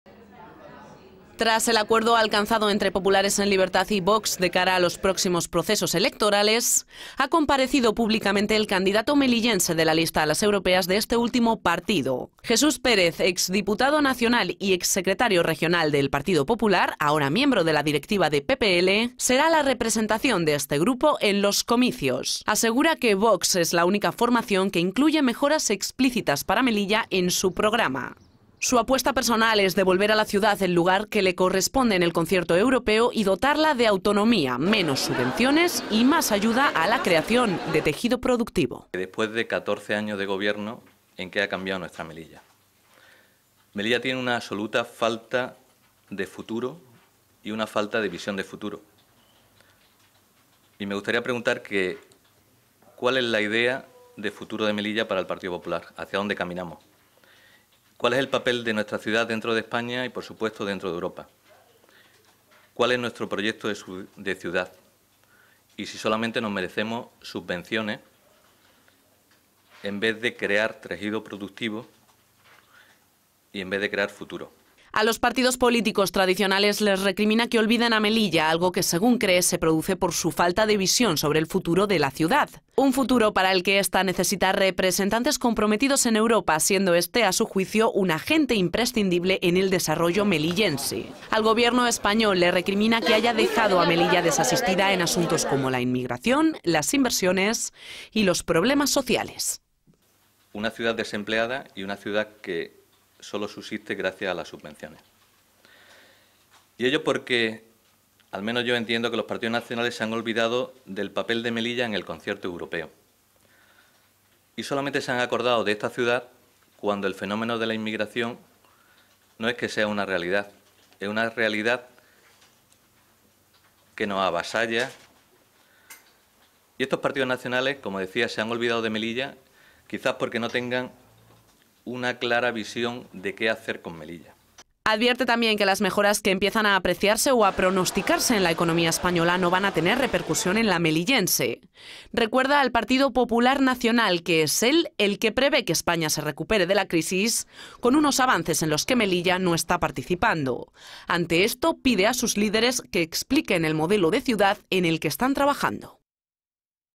Jesús Pérez presentado oficialmente como candidato europeo por el partido VOX
Jesús Pérez, candidato número 12 en la lista electoral de VOX a las elecciones europeas, ha dado hoy su primera rueda de prensa como tal. Este miembro de la directiva de Populares en Libertad ha explicado qe concurre con VOX fruto del acuerdo entre ambos partidos. Este último, asegura, es el único que se ha preocupado de incluir demandas para Melilla en su programa electoral, ofreciendo una alternativa de futuro y oportunidad al abandono al que la someten, advierte, los grandes partidos nacionales.

Tras el acuerdo alcanzado entre Populares en Libertad y VOX de cara a los próximos procesos electorales, ha comparecido públicamente el candidato melillense de la lista a las europeas de este último partido. Jesús Pérez, ex diputado nacional y ex secretario regional del PP, ahora miembro de la directiva de PPL, será la representación de este grupo en los comicios. Asegura que VOX es la única formación que incluye mejoras explícitas para Melilla en su programa. Su apuesta personal es devolver a Melilla el lugar que le corresponde en el concierto europeo y dotarla de autonomía: menos subvenciones y más ayuda a la creación de tejido productivo.

A los partidos políticos tradicionales les recrimina que olviden a Melilla, algo que, según cree, se produce por su falta de visión sobre el futuro de la ciudad. Un futuro para el que la ciudad necesita representantes comprometidos en Europa, siendo este, a su juicio, un agente imprescindible en el desarrollo melillense. Al Gobierno español le recrimina que haya dejado a Melilla desasistida en asuntos como la inmigración, las inversiones y los problemas sociales.

Advierte también que las mejoras que empiezan a apreciarse o a pronosticarse en la economía española no van a tener repercusión en la melillense. Recuerda al Partido Popular nacional que es él el que preve que España se recupere de la crisis con unos avances en los que Melilla no está participando. Ante esto, pide a sus líderes que expliquen el modelo de ciudad en el que están trabajando.